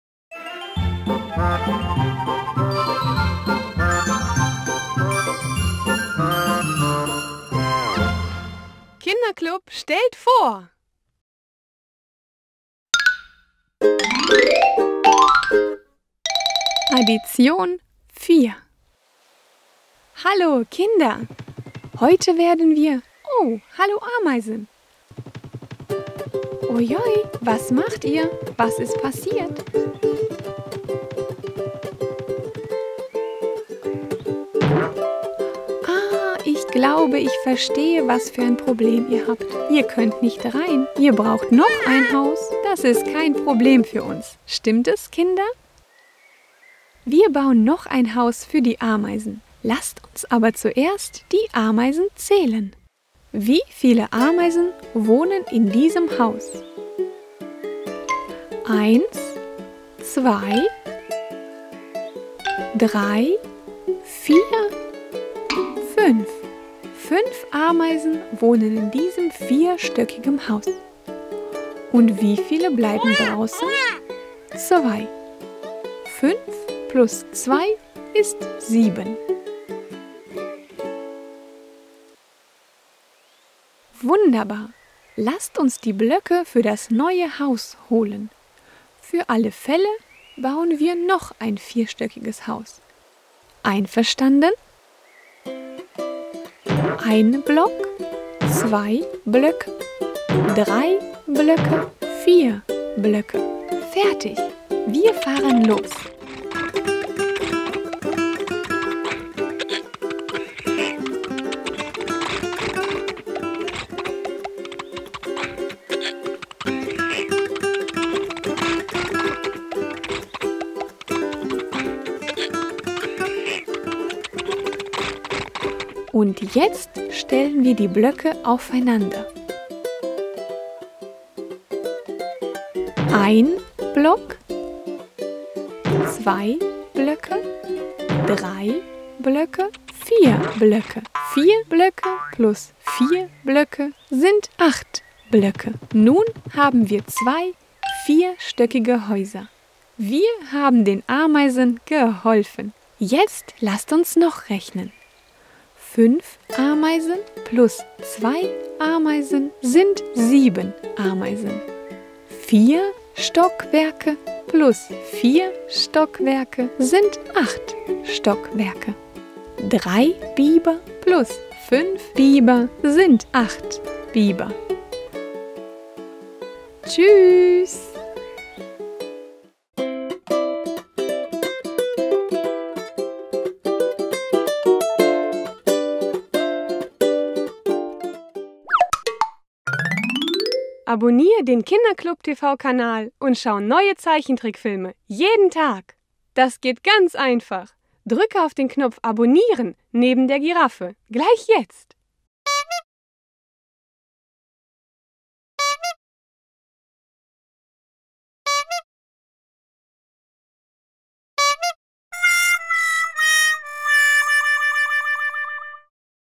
Der Rechenplatz. Addition 4.
Die Ameisen brauchen noch ein Haus. Wer helfen ihnen und bauen ein Haus mit 4 Stockwerken. Dabei zählen wir die Ameisen, Stockwerke, Blöcke und Bieber. :-)